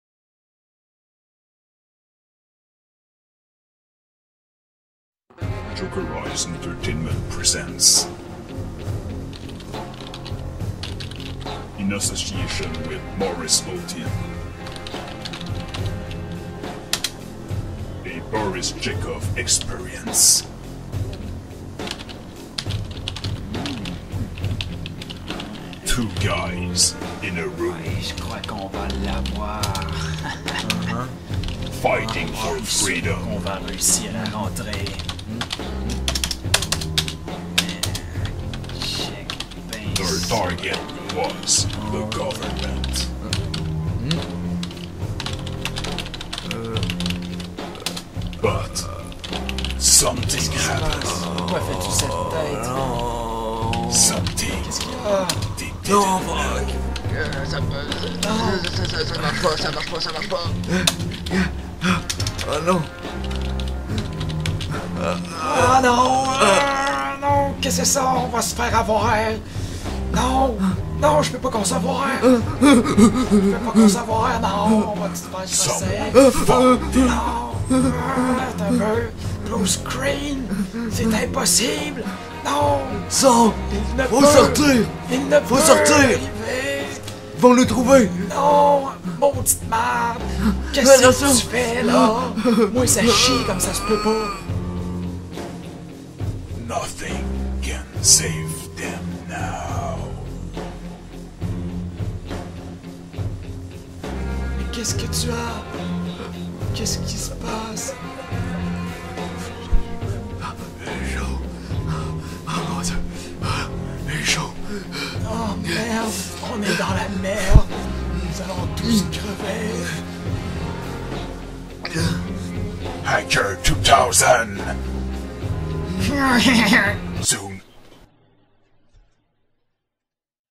Hacker 2000
2 hackers font un ultime essai pour pirater le gouvernement quand tout à coup...